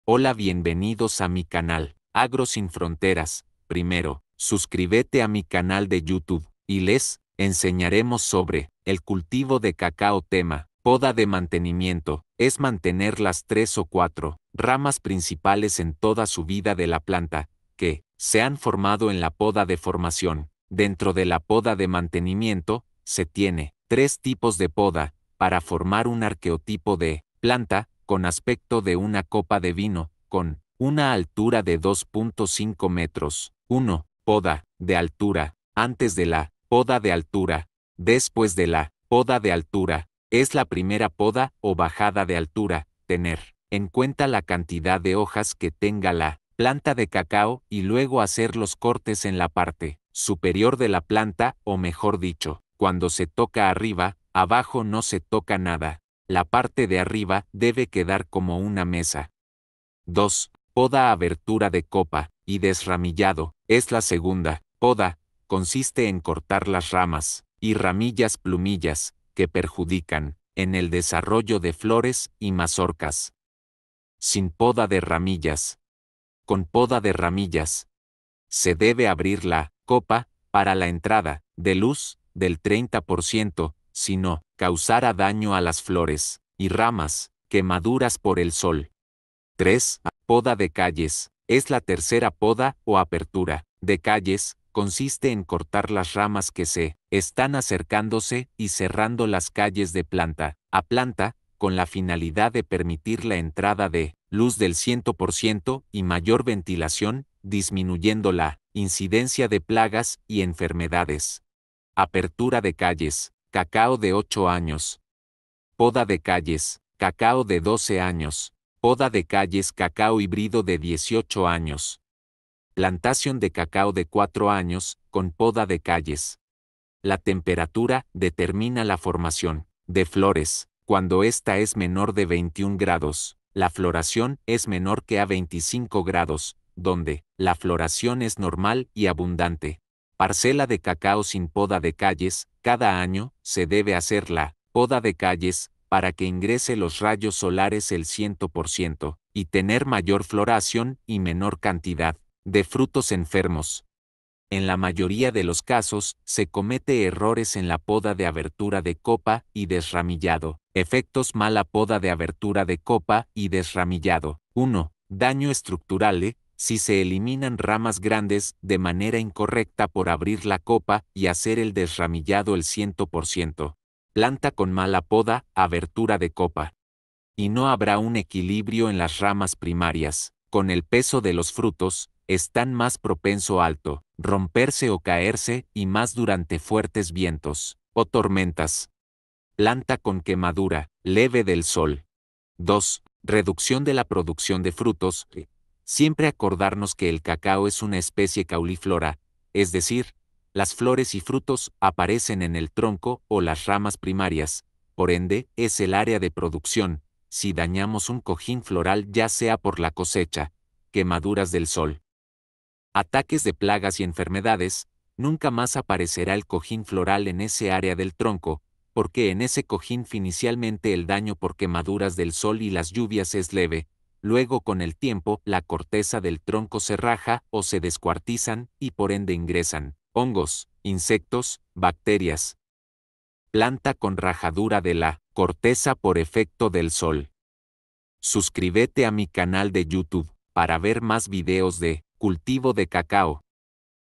PODA DE MANTENIMIENTO EN CACAOTALES EN PRODUCCION (Poda de altura, poda de aclareo y poda de calles)
La poda de mantenimiento en el cacao es una práctica agrícola que se realiza para mantener y mejorar la salud, productividad y calidad de los árboles de cacao. Algunos de los objetivos de la poda de mantenimiento en el cacao son:

Eliminación de ramas muertas, enfermas o dañadas: Estas ramas pueden ser un foco de enfermedades o plagas, por lo que es importante eliminarlas para evitar su propagación y mantener la salud de los árboles.

Control del crecimiento: La poda de mantenimiento también se utiliza para controlar el crecimiento de los árboles de cacao, evitando que se vuelvan demasiado altos o densos. Esto facilita la recolección de los frutos y permite una mejor circulación del aire y la luz, lo que reduce el riesgo de enfermedades y mejora la calidad de los granos de cacao.

Formación de la copa: Mediante la poda de mantenimiento se puede dar forma a la copa de los árboles de cacao, eliminando ramas cruzadas o que se entrelazan, y favoreciendo un crecimiento equilibrado y una distribución uniforme de las ramas.

Renovación de ramas productivas: En algunos casos, la poda de mantenimiento puede incluir la eliminación selectiva de ramas viejas y poco productivas para favorecer el crecimiento de nuevas ramas jóvenes y más productivas.

Es importante realizar la poda de mantenimiento en el momento adecuado, generalmente durante la temporada seca, y utilizar herramientas limpias y afiladas para evitar dañar los árboles. Además, es recomendable contar con conocimientos técnicos o asesoramiento de expertos en cacao para realizar una poda efectiva y beneficiosa para los árboles.